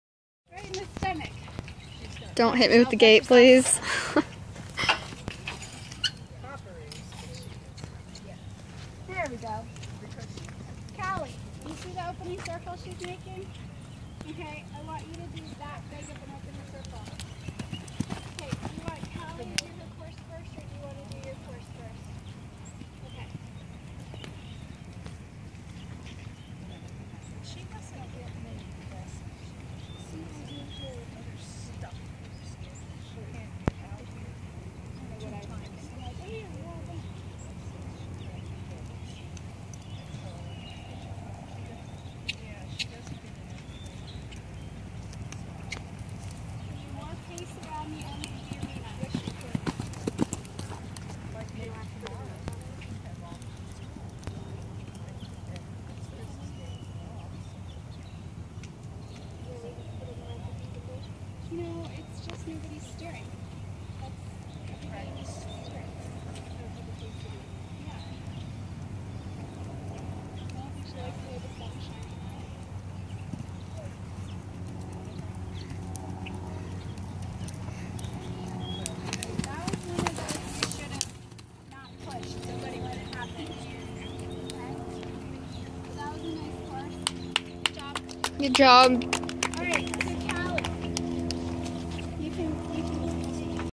Cassidy and Teddy in Short Stirrup Equitation O/F
1st place their first time out jumping a regular course. Desert Trails Schooling Show, January 2015.